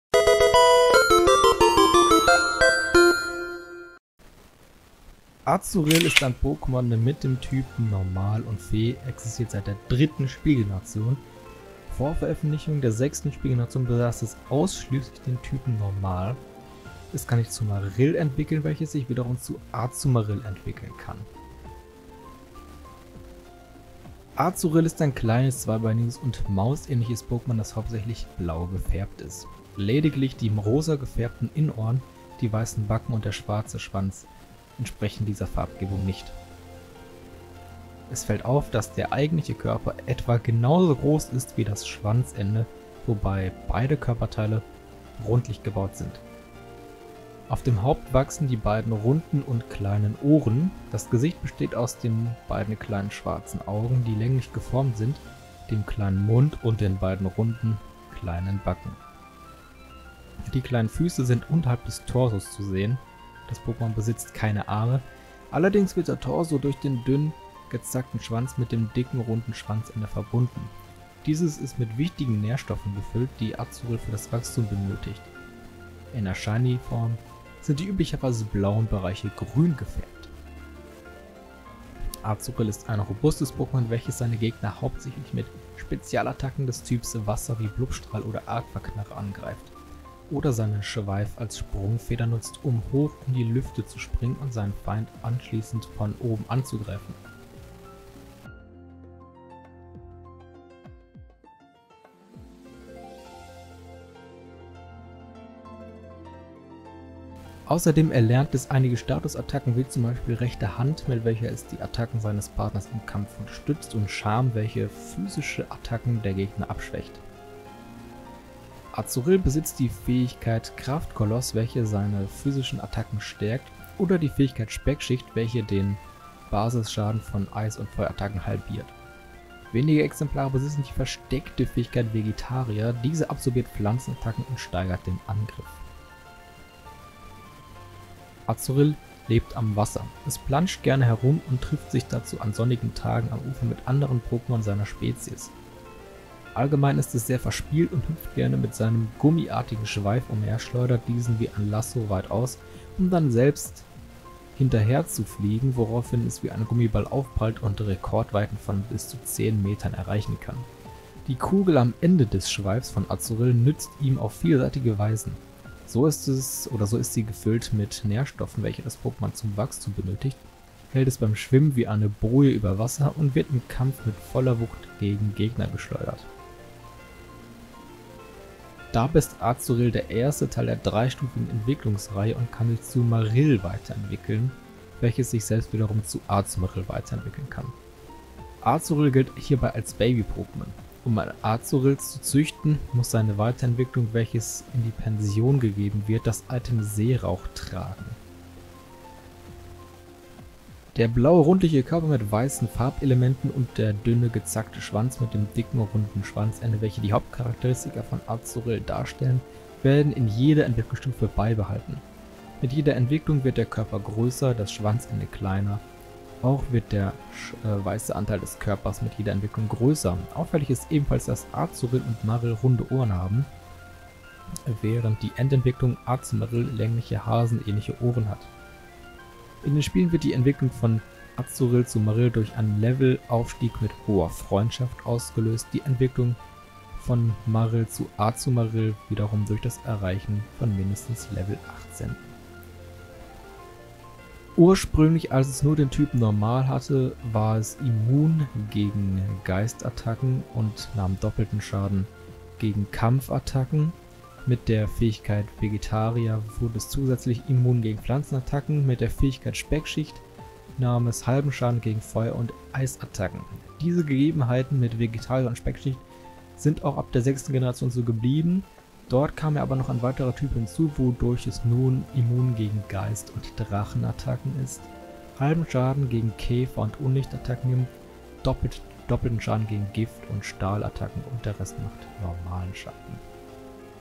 Das Pokémon #298 | Azurill
▶️Worum gehts in diesem Video:

Ich stelle euch das Pokémon Azurill vor. Dabei schauen wir auf das Aussehen, Attacken, Verhalten, Lebensraum, Entwicklung und Typ Schwächen. Viel Spaß :D

0:00 Intro
0:23 Aussehen
1:21 Attacken & Fähigkeiten
2:18 Verhalten & Lebensraum
2:58 Entwicklung
4:04 Typ Schwächen